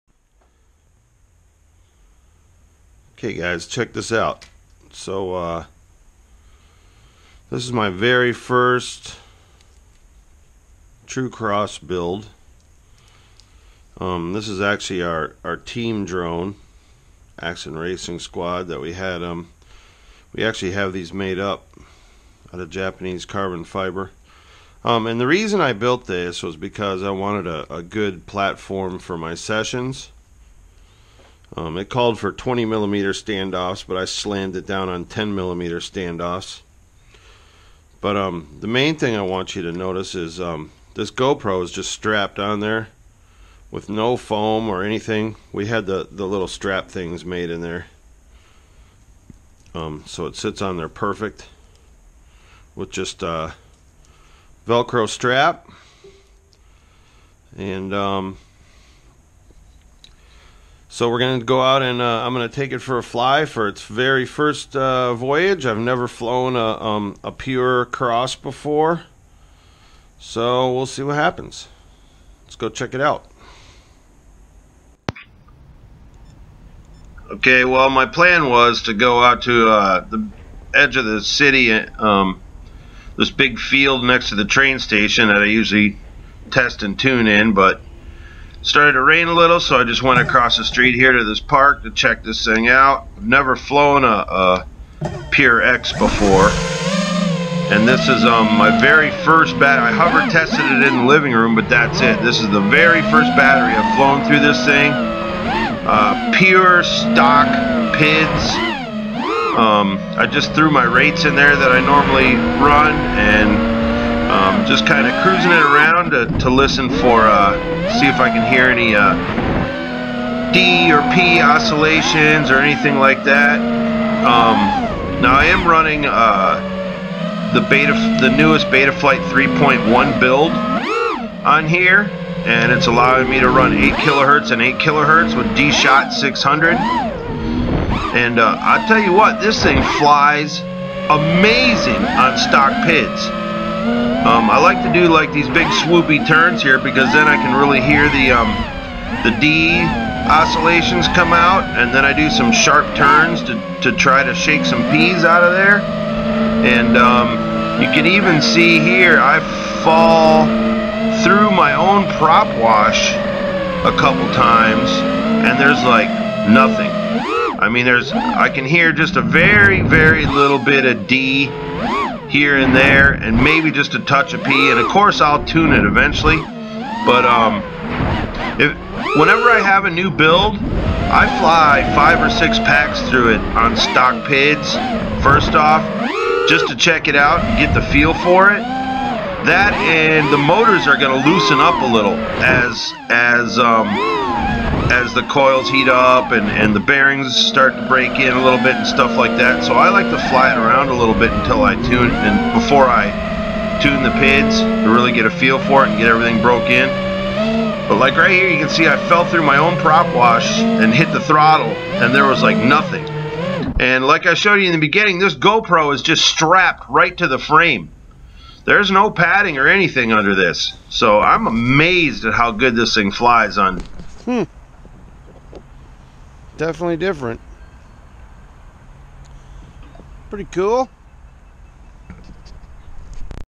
New build..stock PIDs..Wow! The newest unreleased betaflight 3.1
Running 8 khz and 8khz with Dshot 600 on an F3 board!
Xracer v3.1 FC
ZTW flash 20amp esc's
T-motor F40's
King Kong 5x4x3 props